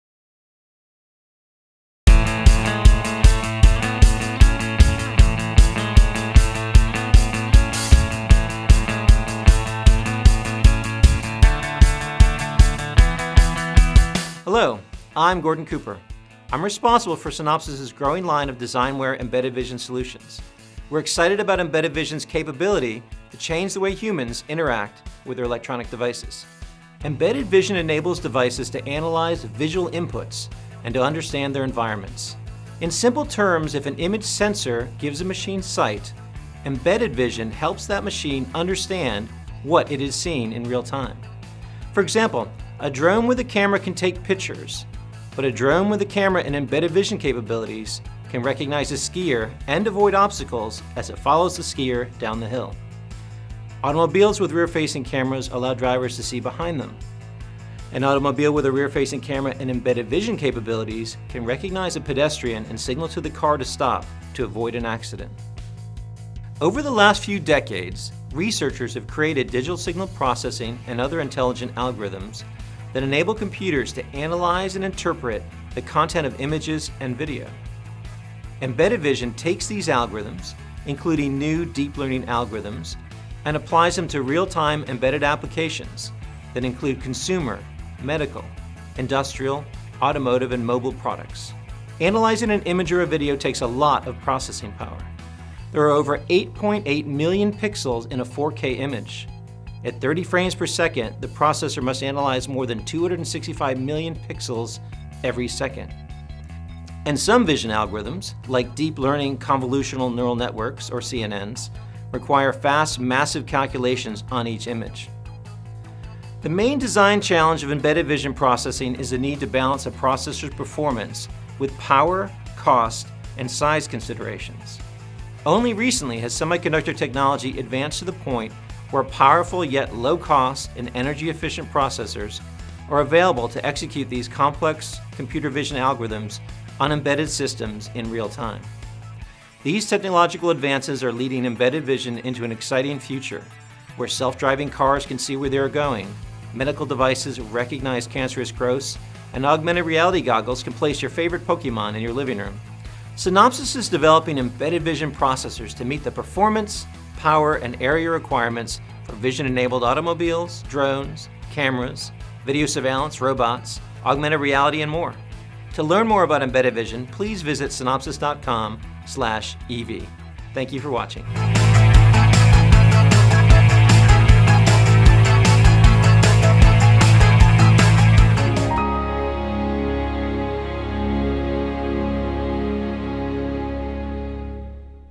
Introduction to Embedded Vision | Synopsys
This video is your starting point for learning about embedded vision. It provides an overview of embedded vision and starts the conversation on how embedded vision is changing the way humans interact with their electronic devices.